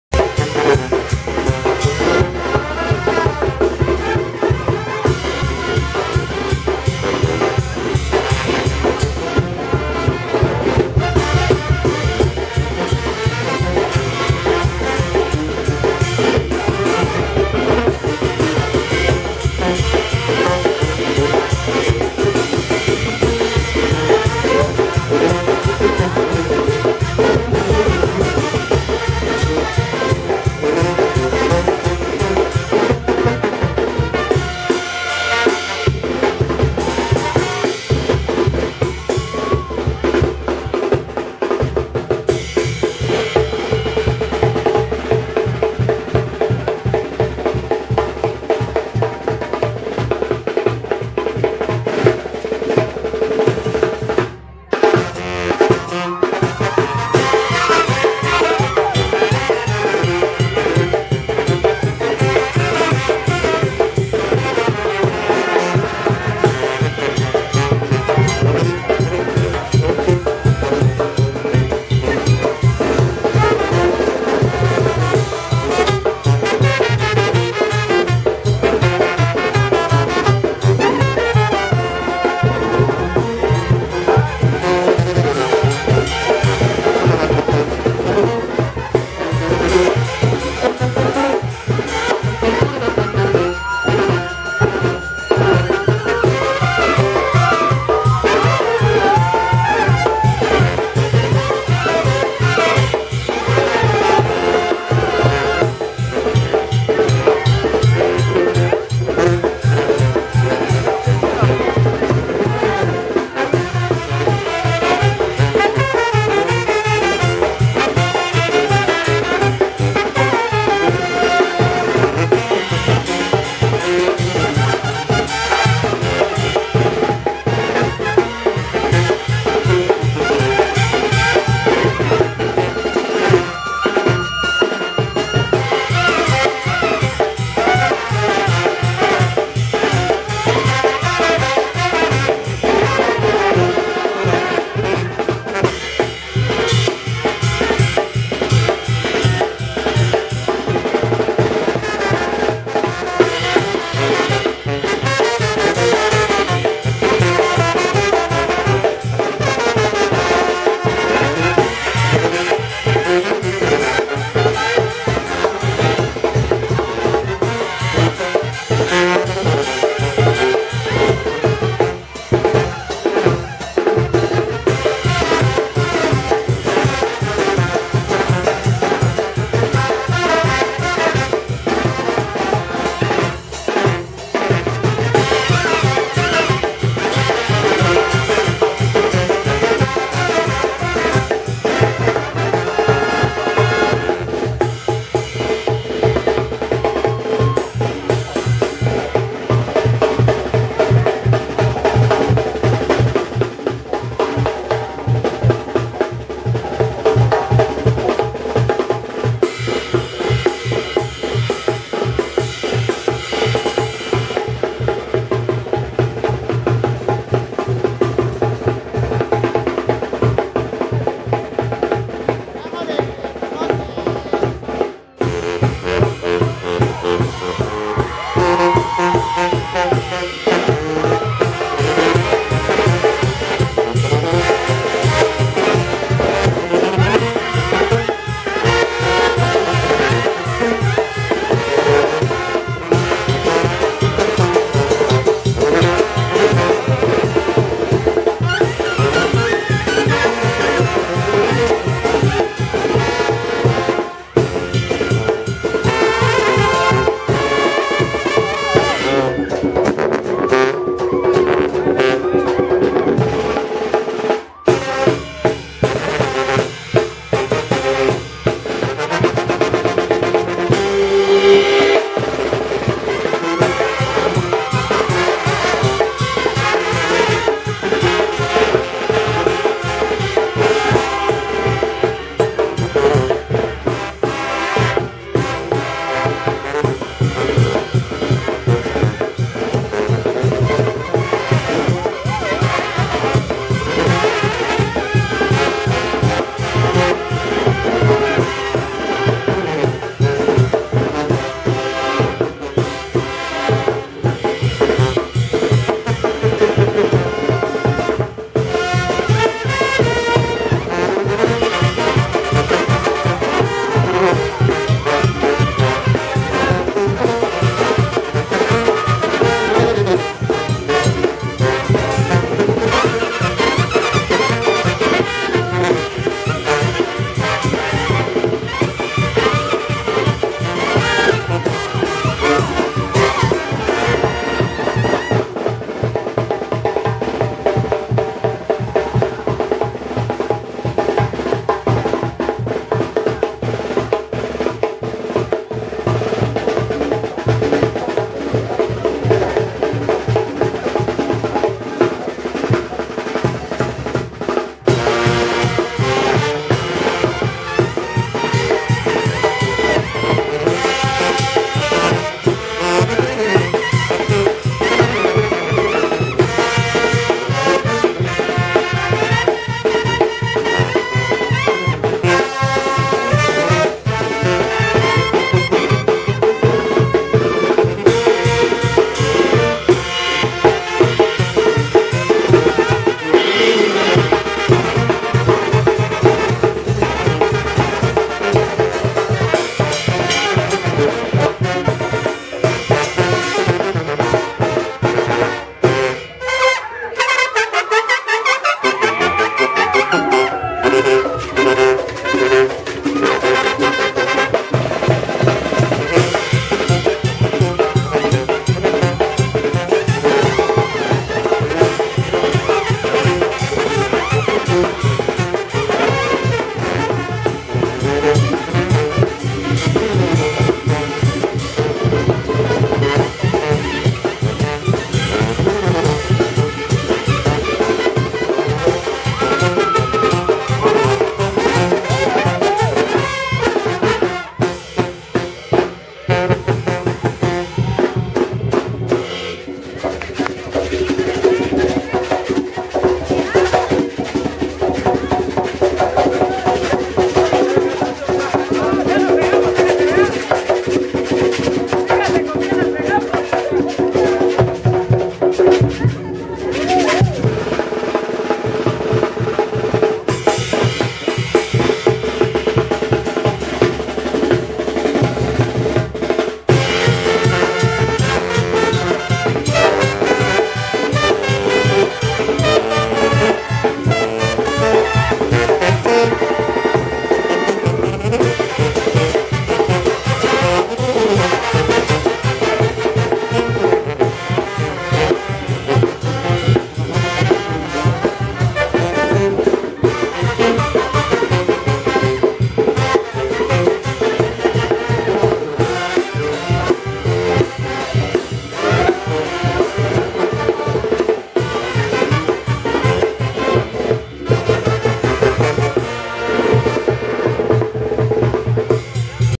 Soledad Etla-Muerteada femenil 2016-Las Catrinas-Majestuoso Barrio D Arriba-Banda Reyna De Huajuapan
Banda Dominguera-Tiliches-Clasicumbias esto en el Mjestuoso Barrio De Arriba de Soledad Etla Oaxca.Y NO TE ACABES MI REYNA